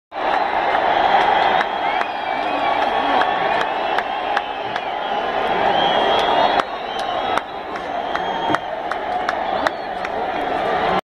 A B De Villiers in India
First time a foreign player got such welcome in India...a true champion...